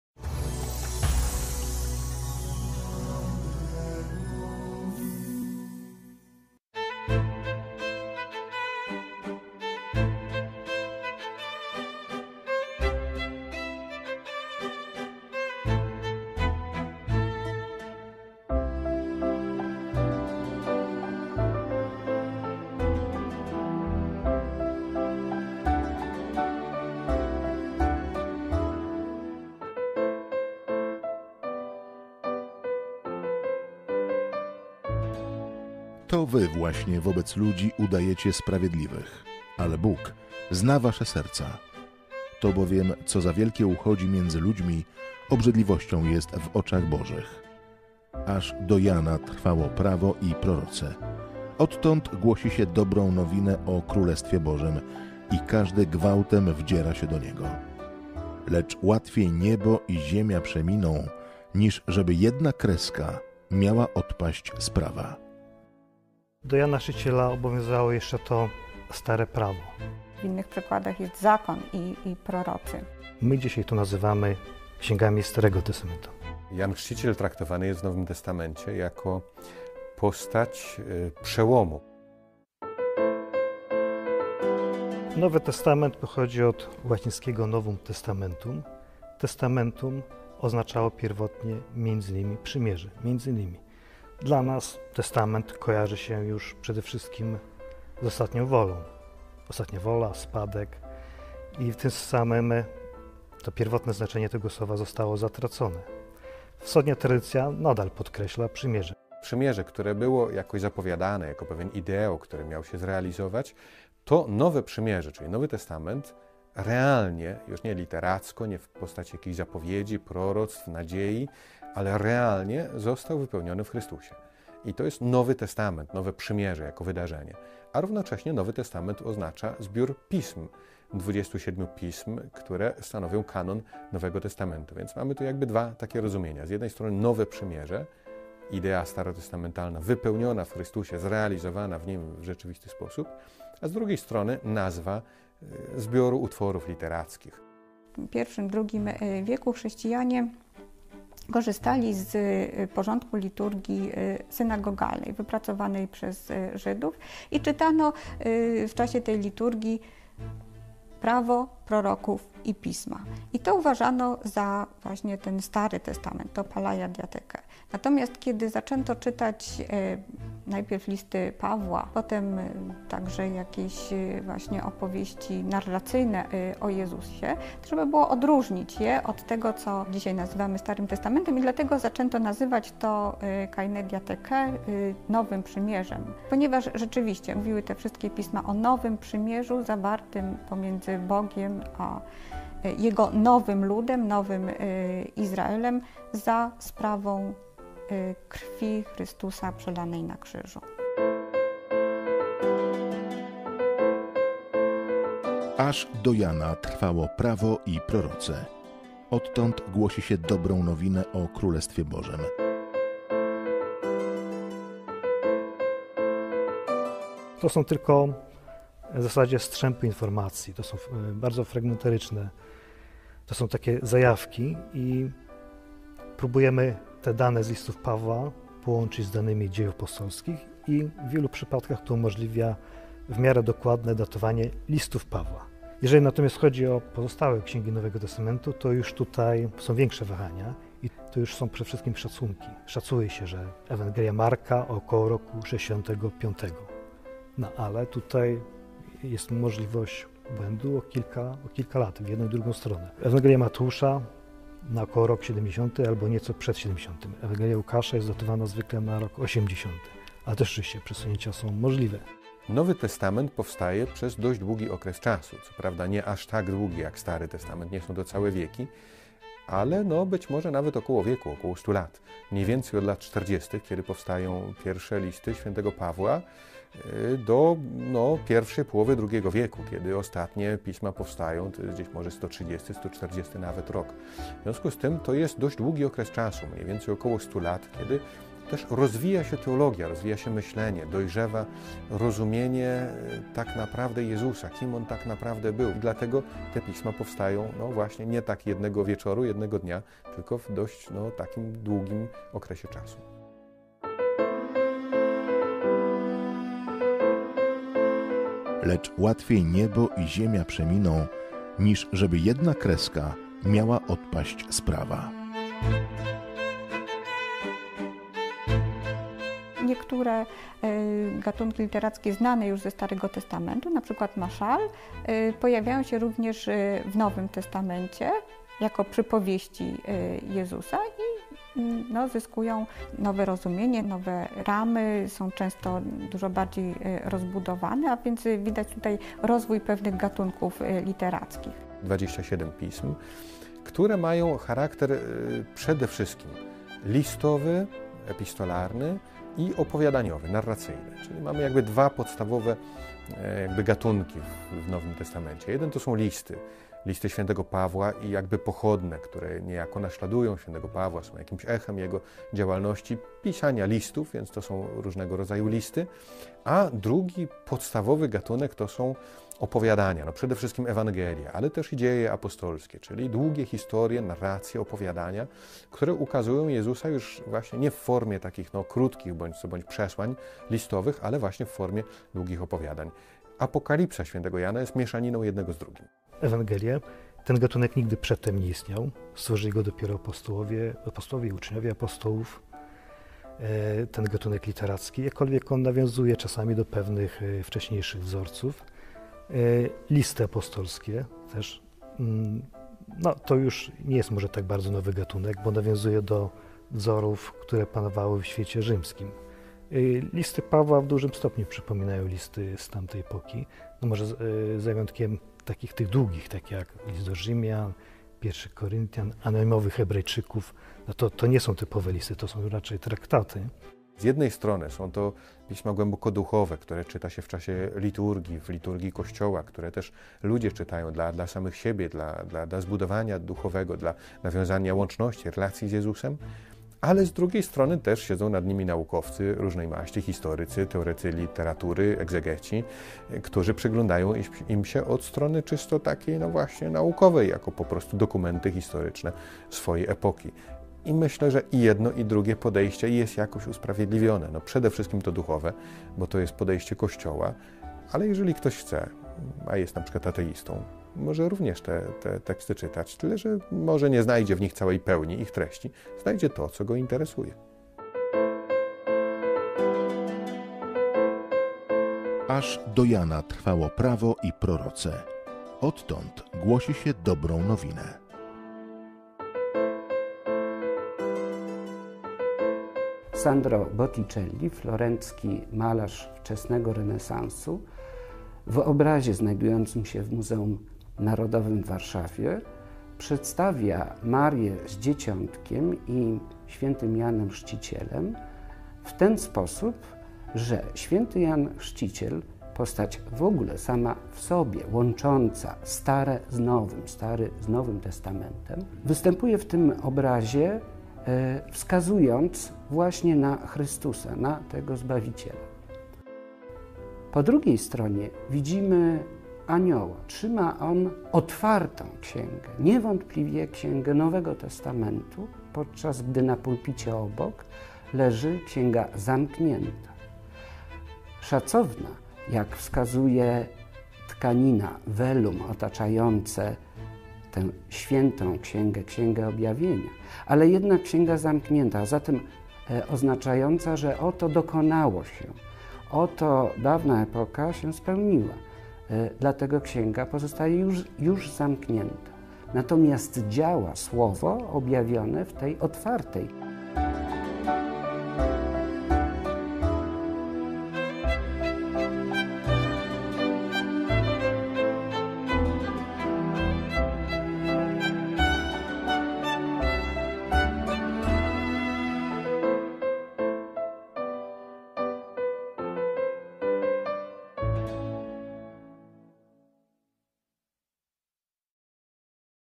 Biblijna lekcja religii. Odc. 1 Co to jest Nowy Testament ?
Nowy cykl rozważań biblijnych rozpoczynają na pozór oczywiste pytania: co to jest Nowy Testament? Czy jednak jest ono tak oczywiste? Czy wiemy, kto i kiedy wprowadził tę nazwę? Dlaczego właśnie tę? Z jakich ksiąg się składa? A jak Nowy Testament i rozpoczynającą go nową erę przedstawiają artyści?
Z Ewangelii według Świętego Łukasza :
Powiedział więc do nich: "To wy właśnie wobec ludzi udajecie sprawiedliwych, ale Bóg zna wasze serca. To bowiem, co za wielkie uchodzi między ludźmi, obrzydliwością jest w oczach Bożych.
 Aż do Jana sięgało Prawo i Prorocy; odtąd głosi się Dobrą Nowinę o królestwie Bożym, i każdy gwałtem wdziera się do niego.Lecz łatwiej niebo i ziemia przeminą, niż żeby jedna kreska miała odpaść z Prawa.